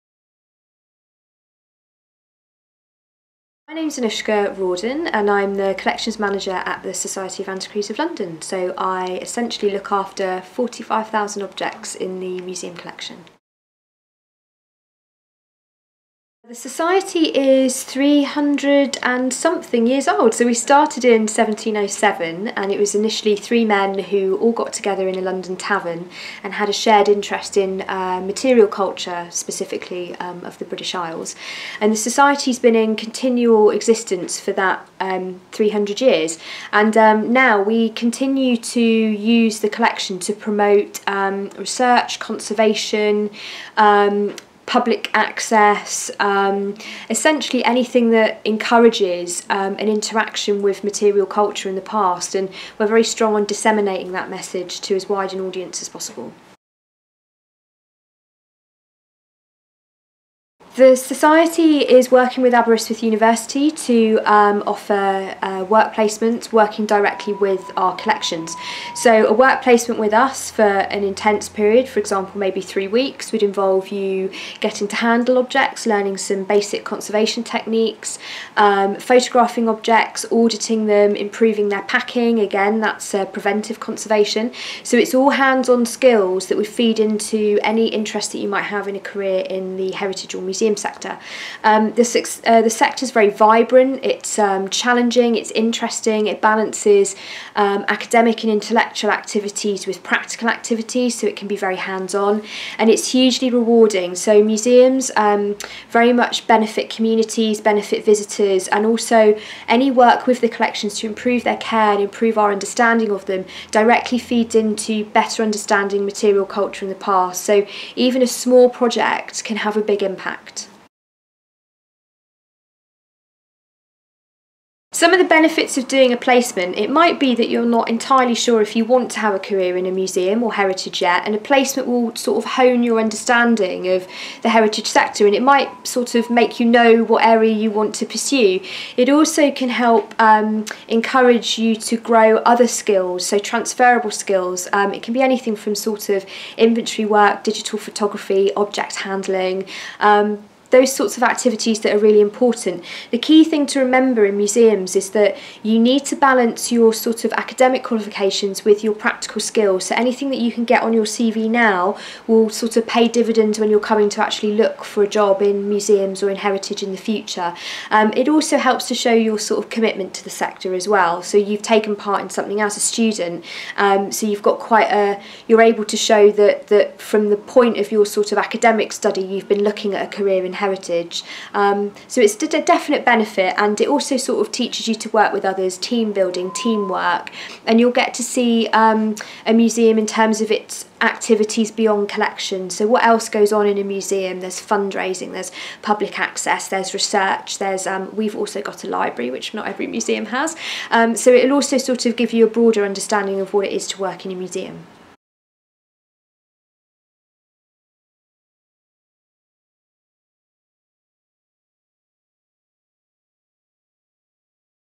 Thinking of a Career in Heritage?
Anooshka Rawden, Collections Manager at the Society of Antiquaries of London, came to Aberystwyth to speak with our students about the Heritage Sector Student Placements they run with the Department of History and Welsh History.